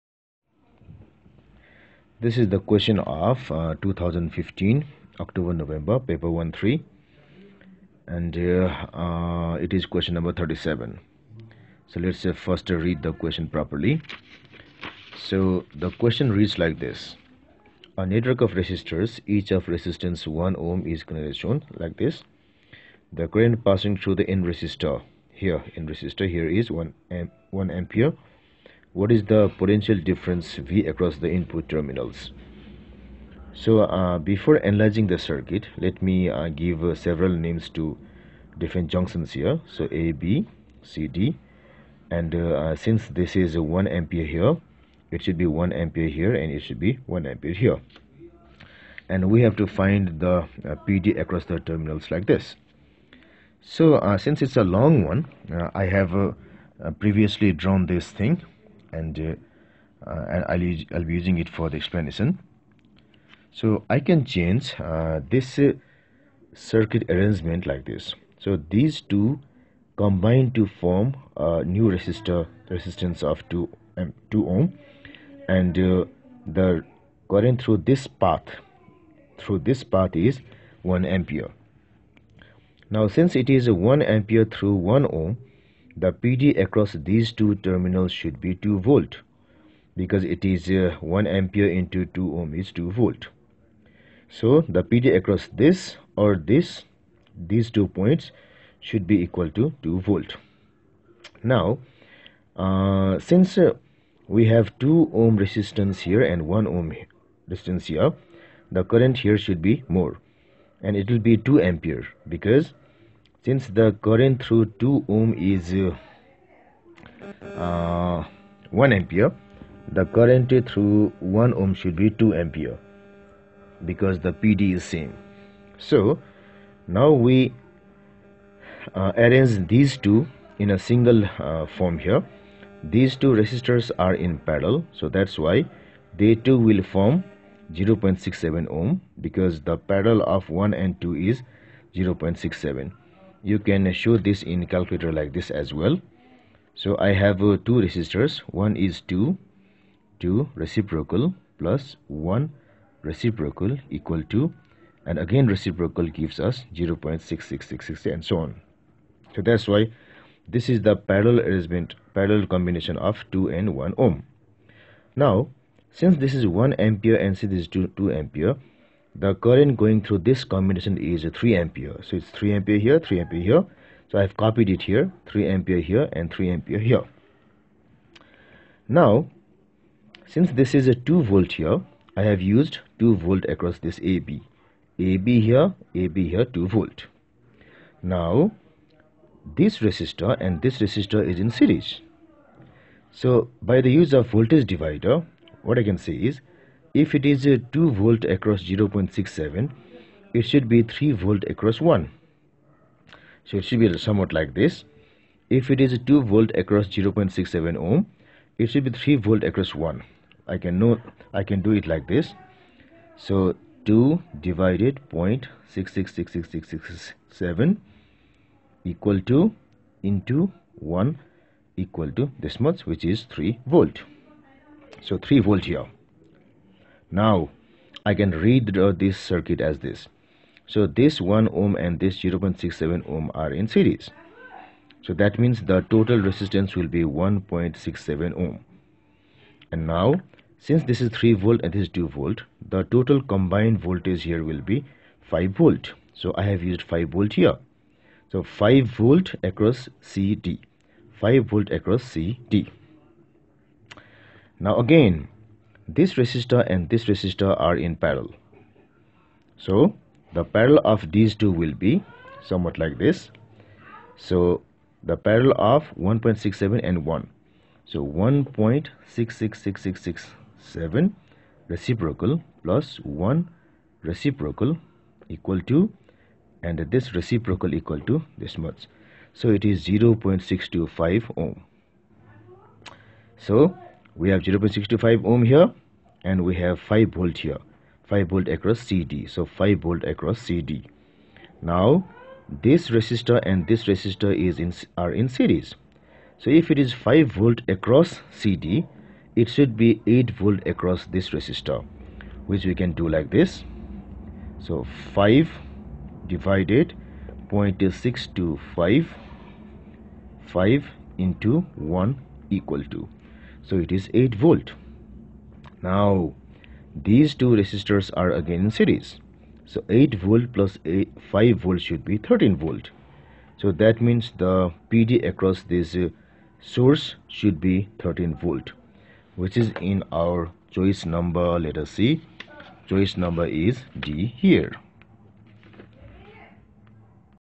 2015 CAIE AS & A level October November Physics Paper 13 Q N 37 (9702/13/O/N/15) by Sajit C Shakya
2015 Cambridge International AS & A level October November Physics Paper 13 Q. No. 37 (9702/13/O/N/15) by Sajit Chandra Shakya from Nepal

A network of resistors, each of resistance 1Ω, is connected as shown.

The current passing through the end resistor is 1A.

What is the potential difference (p.d.) V across the input terminals?

A 2V B 5V C 8V D 13V